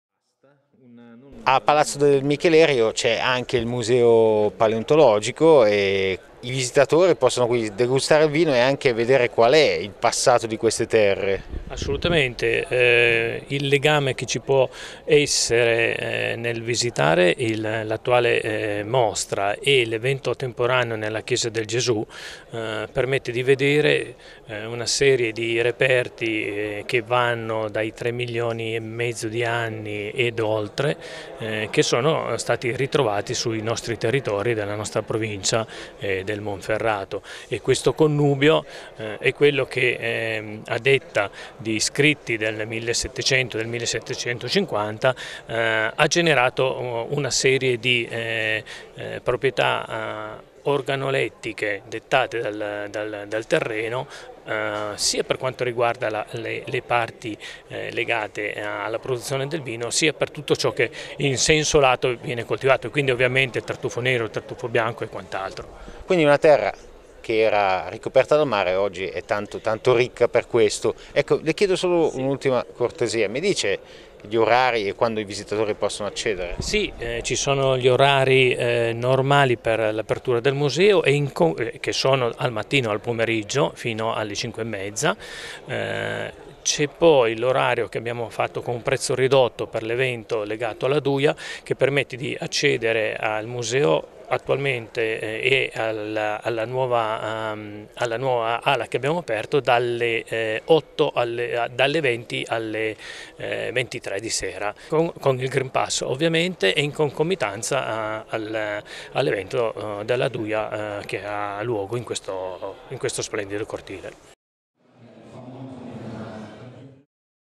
Asti: Alla Douja le balene nate nella terra dei vini - GRP TELEVISIONE
All'interno degli eventi collaterali della Douja d'or 2021, sarà inaugurata venerdì 17 settembre la mostra al museo paleontologico di Palazzo Michelerio delle "Balene preistoriche". Questi meravigliosi mammiferi vivevano nelle terre che poi sono emerse dando vita al Monferrato,  circa tre milioni e mezzo di anni fa. Un connubio quello tra terre di vini e mare non del tutto casuale.